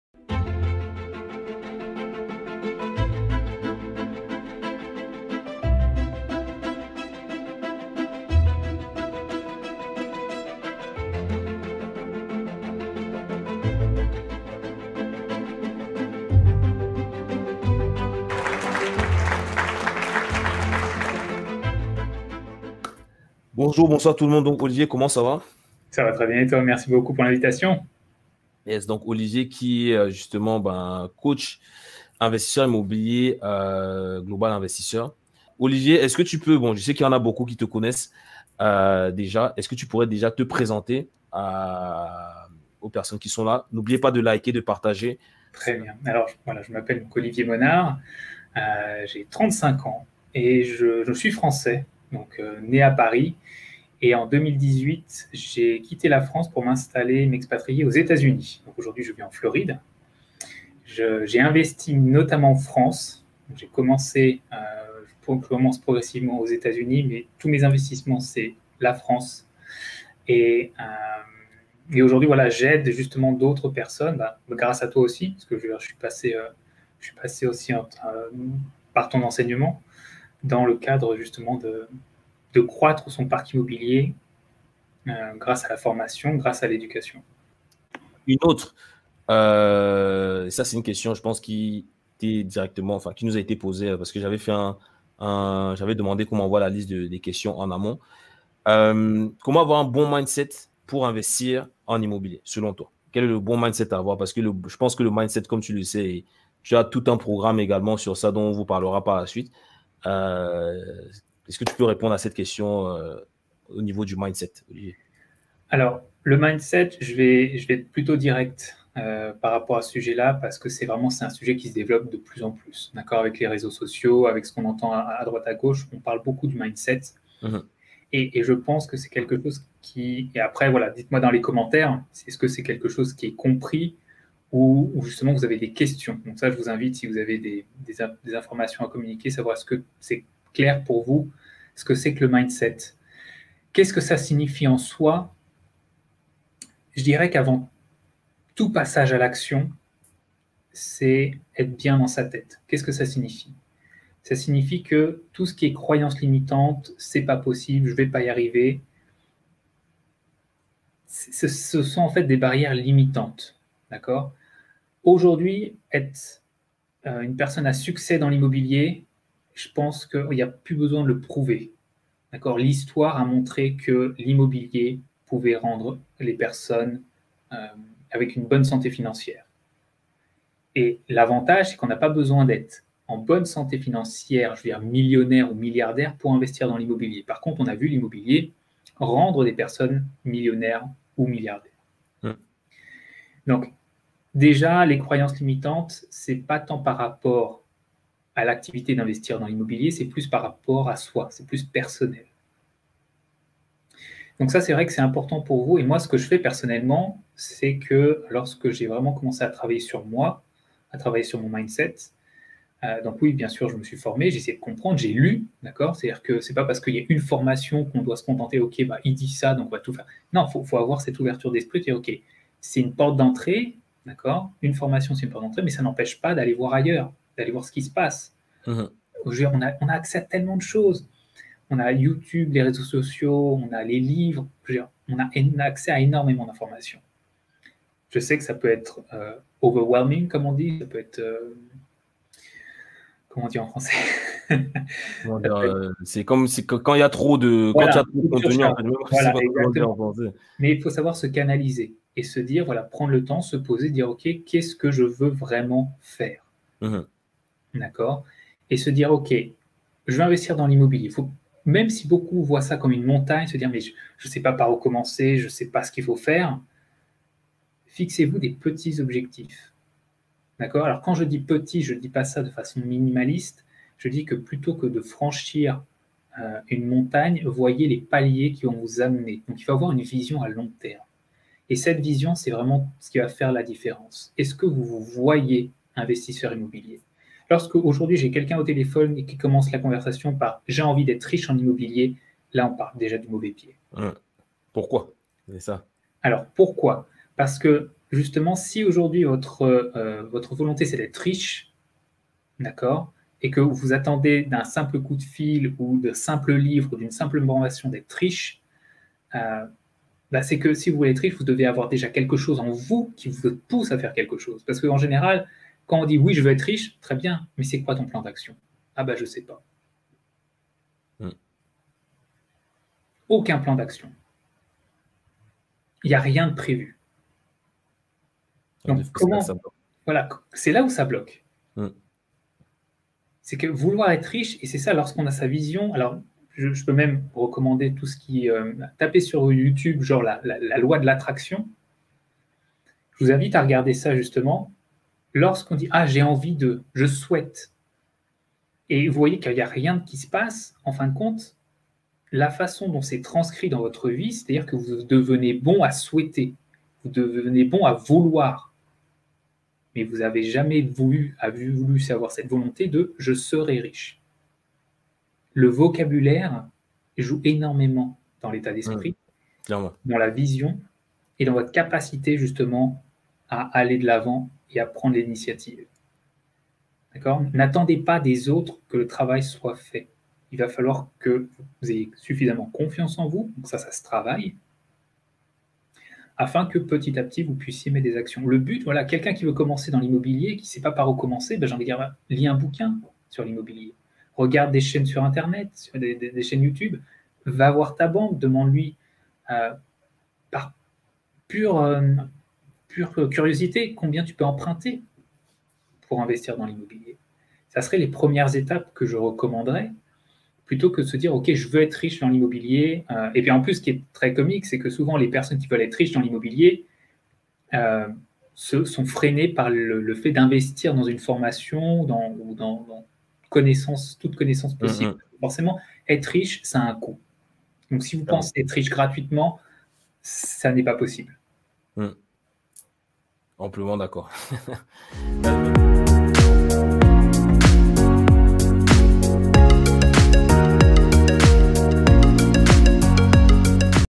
Motivation Pour Réussir En Immobilier - Live Avec Olivier Monard
Dans cette nouvelle vidéo, je donne la parole à Olivier Monard qui est l'un des coachs de l'équipe Global Invest. Avec Olivier nous allons parler de la motivation qui reste indispensable quand on désire investir en immobilier. Le mindset de l'investisseur est tout aussi important pour rester centré sur ses objectifs. Sur une échelle de 1 à 10, je serais curieux de savoir à quel niveau tu te situes, sachant que 10 représente le top de la motivation absolue ! J'attends ta réponse en commentaire ;-)

Motivation Pour Réussir En Immobilier

Avertissement